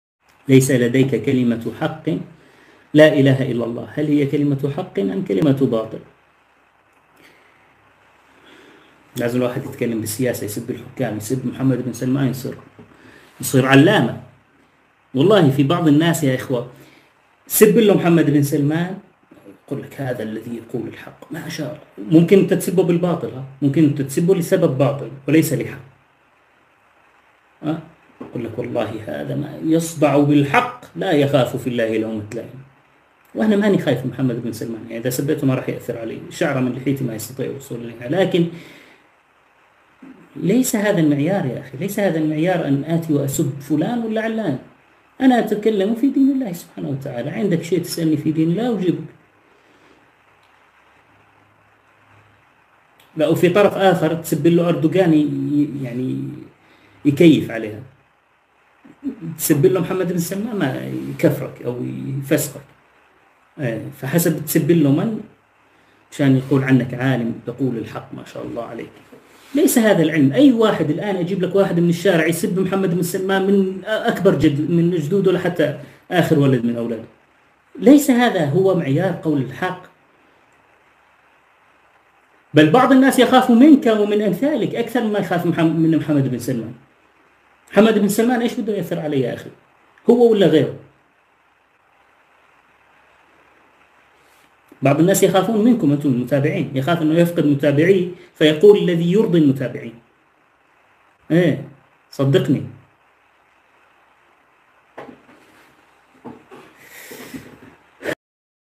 Insulting The Rulers makes you a Scholar for some people - Muhammad bin Shams ad-Deen
Shaykh Muhammad bin Shams ad-Deen